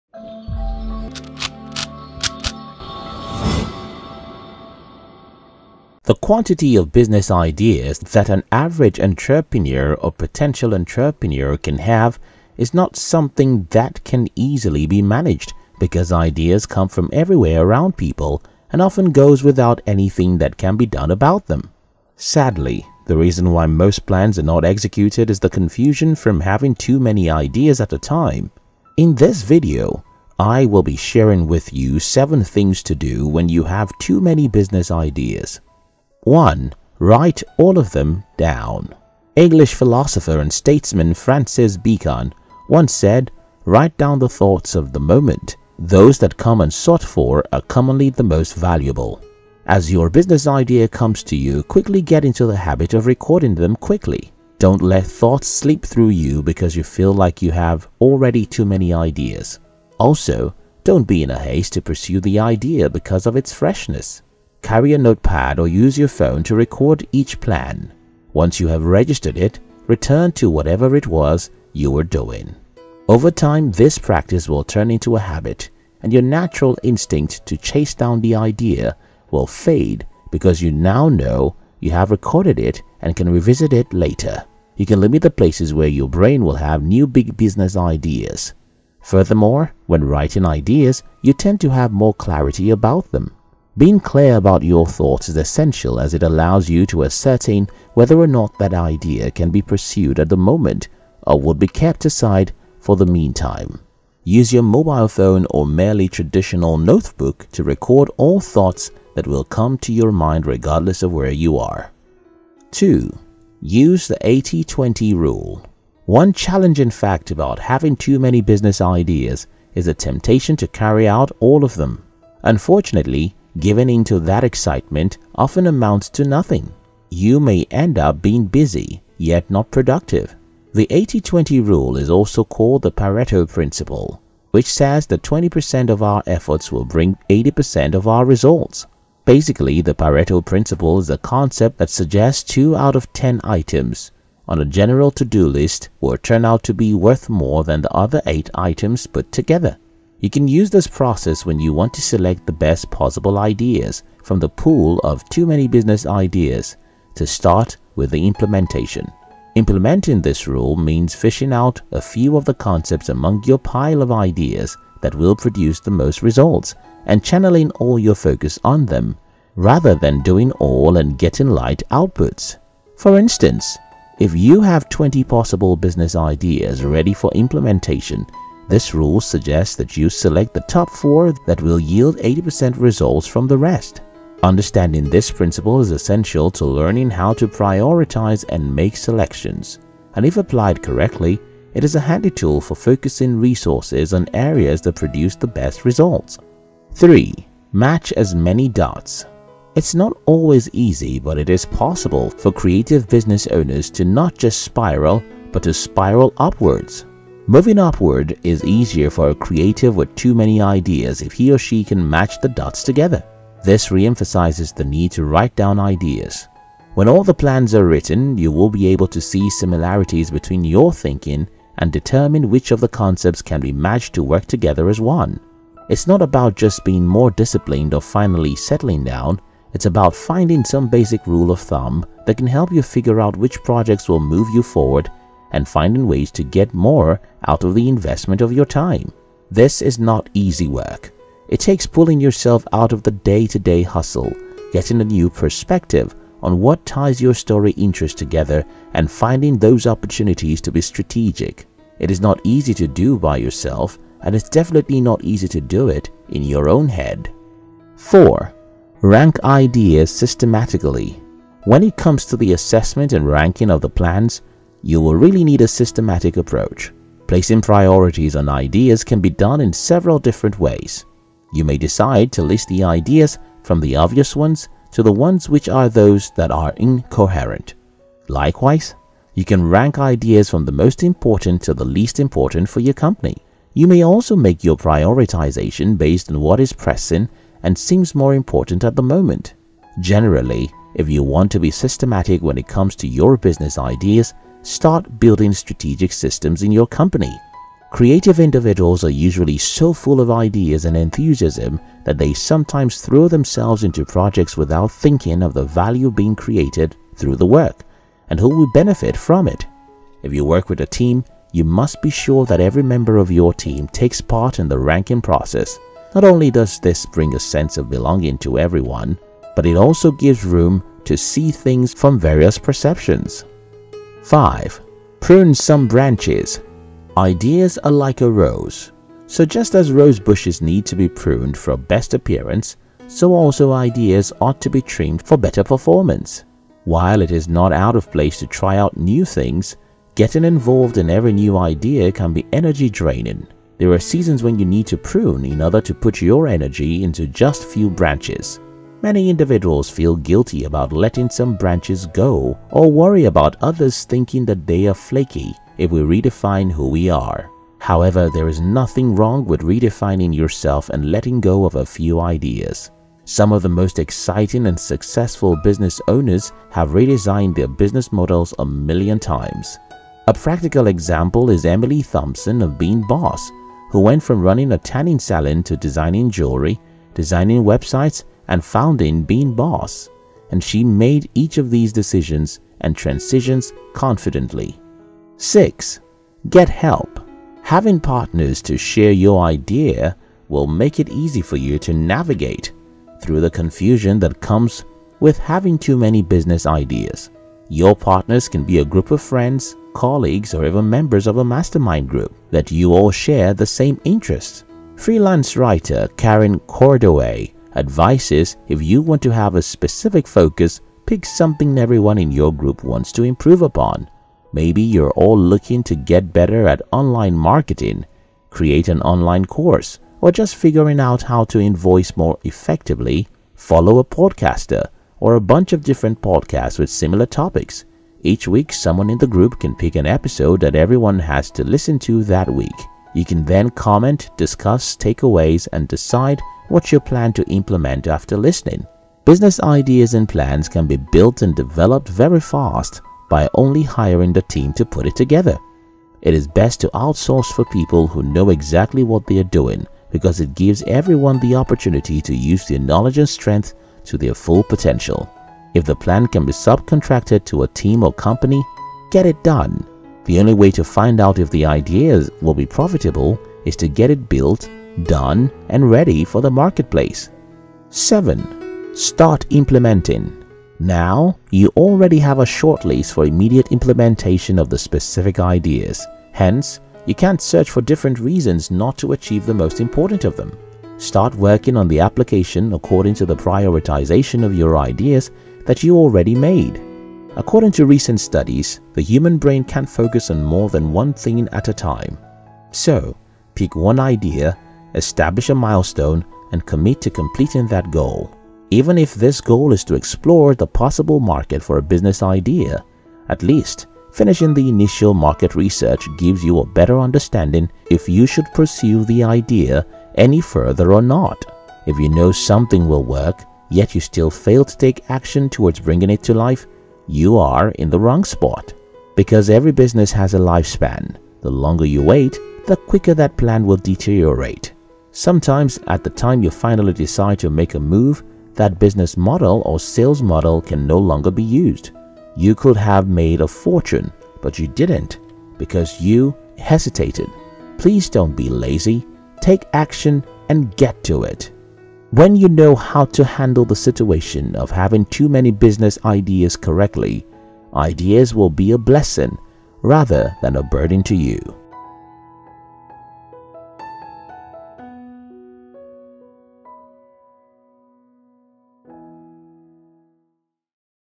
What to do when you have too many business Ideas
The quantity of business ideas that an average entrepreneur or potential entrepreneur can have is not something that can be easily managed because ideas come from everywhere around people and often goes without anything that can be done about them. Sadly, the reason why most plans are not executed is the confusion from having too many ideas at a time.